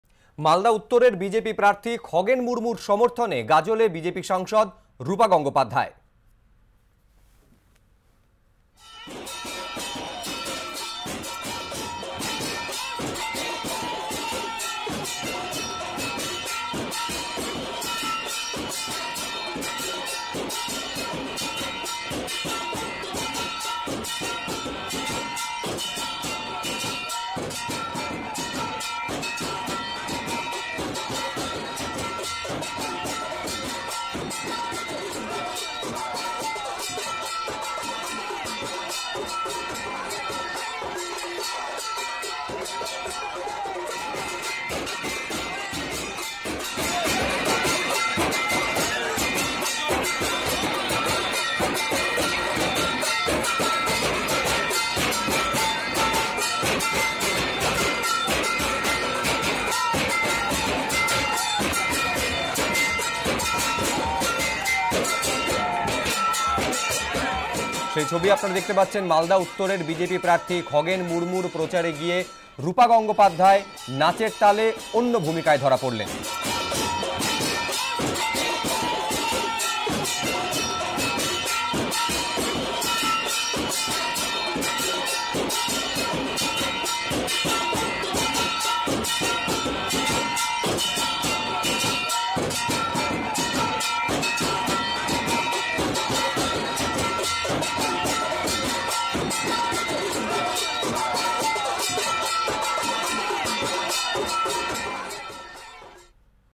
BJP MP Roopa dances with locals in election rally at Malda | ABP Ananda
মালদহ উত্তরের বিজেপি প্রার্থী খগেন মুর্মুর সমর্থনে সাধারণ মানুষের সাথে তালে তালে পা মেলালেন বিজেপি MP রুপা গঙ্গোপাধ্যায় | আরো জানতে দেখতে থাকুন এই খবর |